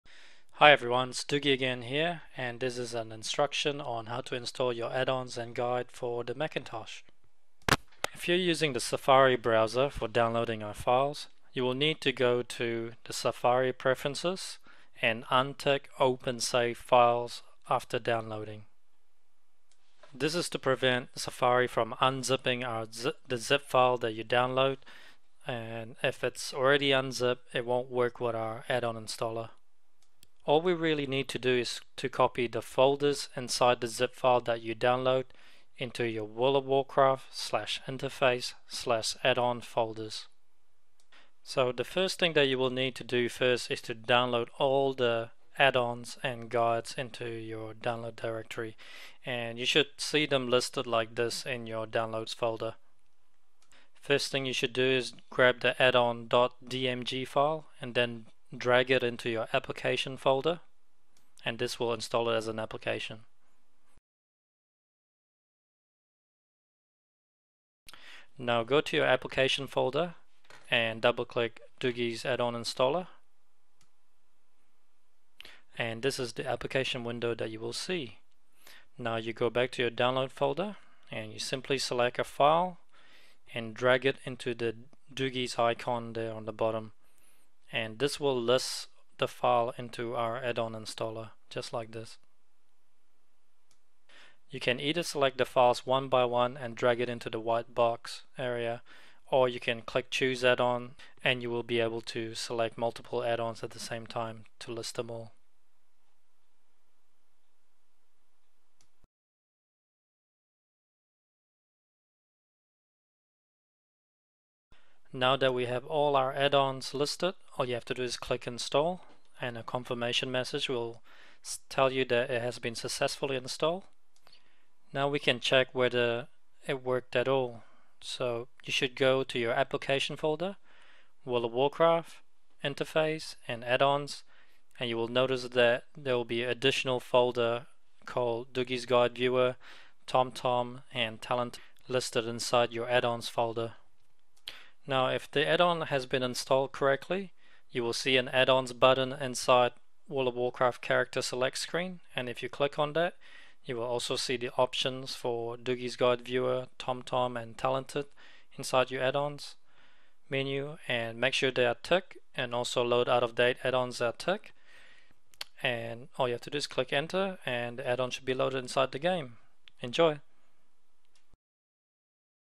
How To Install Addon in Mac - Dugi's Addon Installer
Step by step guide on how to install Dugi Guides addon for Macintosh.